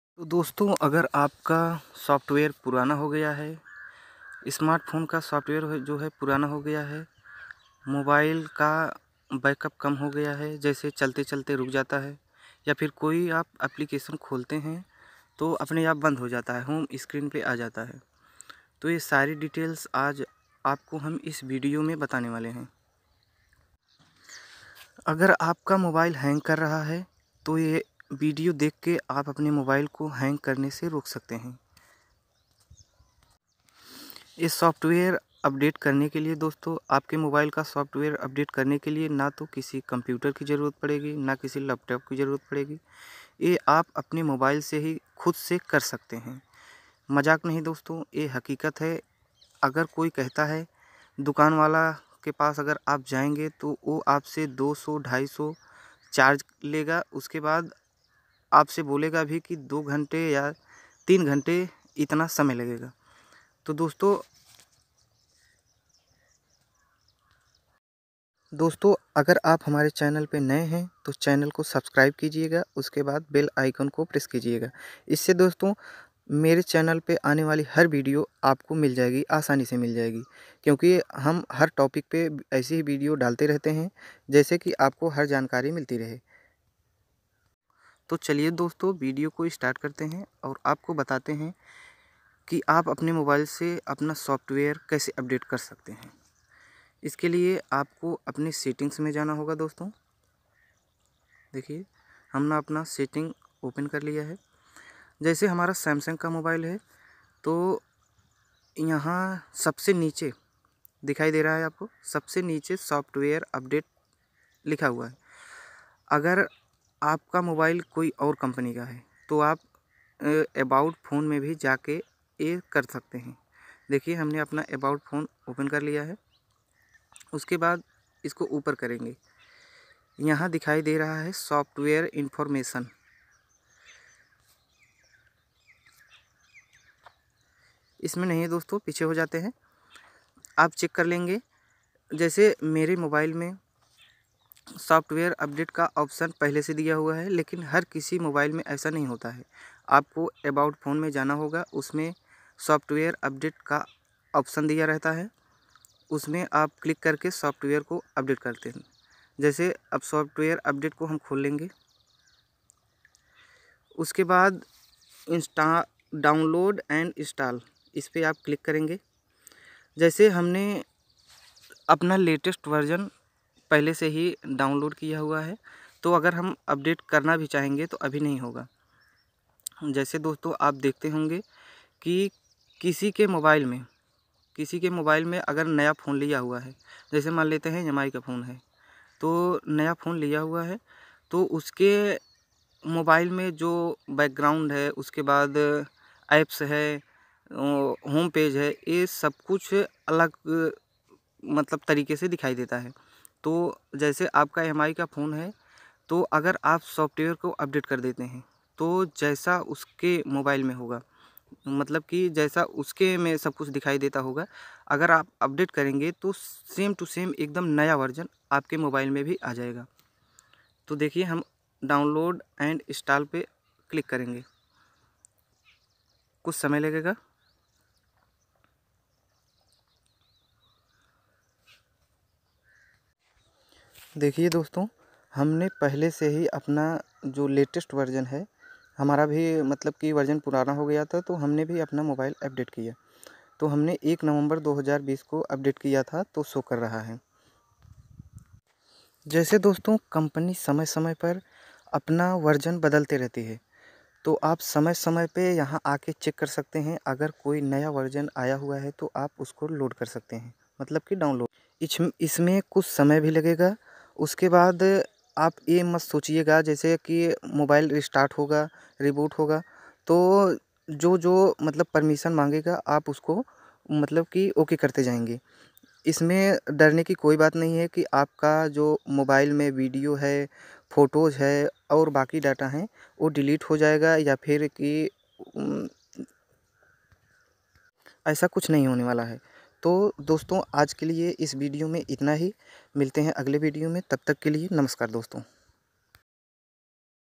How to install software in mobile? Mobile me software kaise dale in hindi ?
Is video me ham apko batayenge ki bina kisi laptop aur cumputer ke apne mobile me aasani se naya software kaise dalte hai smartphone chahe kisi company ka ho koi problem nahi hogi Micromax motorola Samsung intex mi redmi realme Nokia kisi bhi company ka phone ho koi dikkat ya paresaninahi hogi.